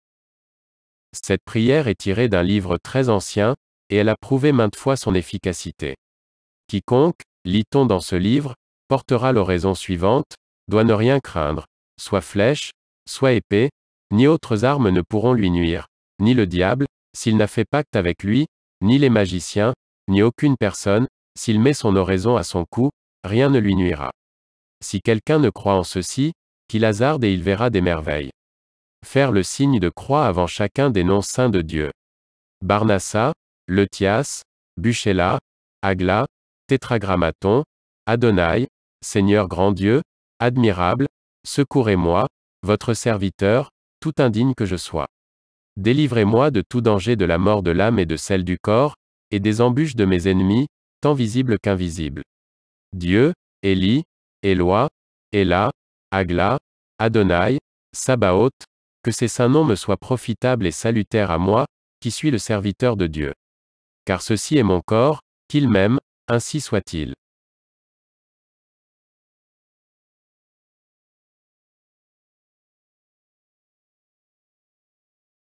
Prière Audio en Français : Oraison de grande protection en vertue de laquelle nul ne peut vous nuir
Cette prière est tirée d'un livre très ancien, et elle a prouvé maintes fois son efficacité.
"Quiconque, lit-on dans ce livre, portera l'oraison suivante, doit ne rien craindre: soit flèches, soit épées, ni autres armes ne pourront lui nuire; ni le diable, s'il n'a fait pacte avec lui, ni les magiciens, ni aucune personne, s'il met son oraison à son coup, rien ne lui nuira. Si quelqu'un ne croit en ceci, qu'il hasarde et il verra des merveilles.

+Barnasa +Leutias +Bucella +Agla +Tetragrammaton + Adonaï
+seigneur, grand dieu, admirable , secourez (votre nom), votre serviteur, tout indigne que je sois. Délivrez moi de tout danger, de la mort de l'âme et de celle du corps, et des embûches de mes ennemis, tant visible qu'invisible.

+Dieu +Ely +Eloy +Ela +Agla +Adonaï +Sabaoth, que ces saint noms me soient profitables et salutaires à moi, (votre nom), qui suis les serviteur de dieu.
+ Car ceci est mon corps,
+qu'il m'aime,
+ainsi soit-il.